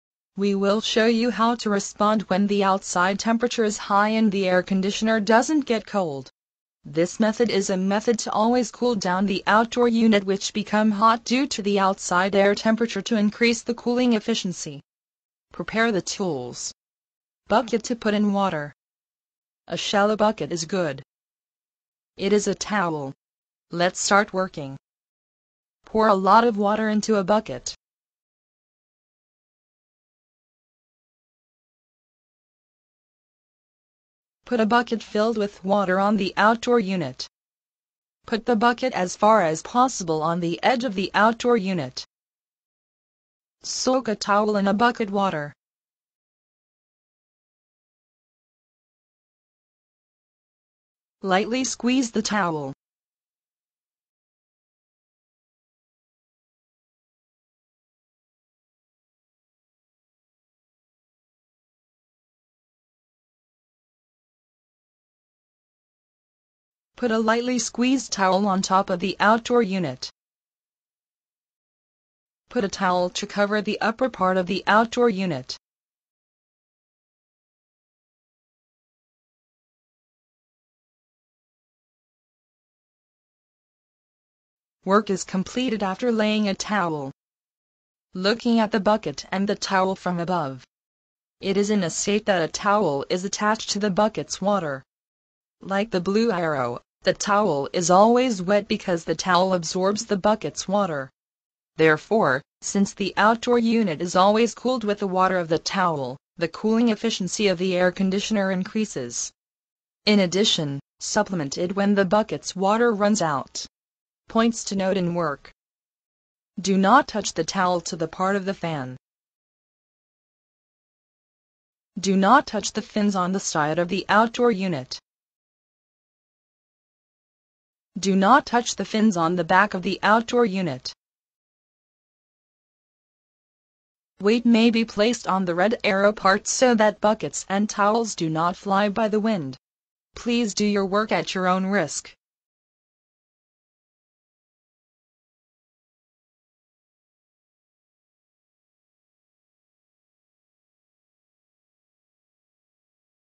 Cool the air conditioner outdoor unit! A trick that dramatically cools even in extreme heat
It is a method of cooling the room temperature dramatically with the air conditioning.
It is a method of lowering indoor temperature when outside temperature is high.
By cooling the outdoor unit of the air conditioner, the capacity of the air conditioner improves.